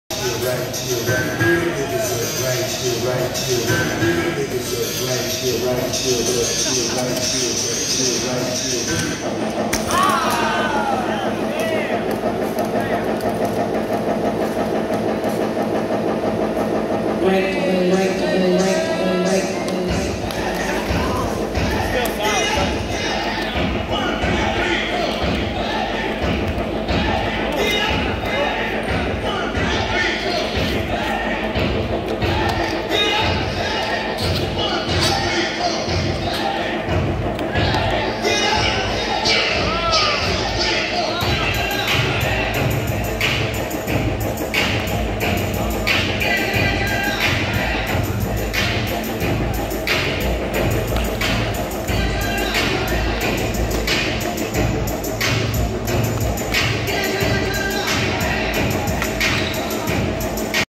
Cuttaz practicin 1 foot toe spins at Skate City Cuttaz practice! 17 Apr19
We still here. Our latest vids on Facebook.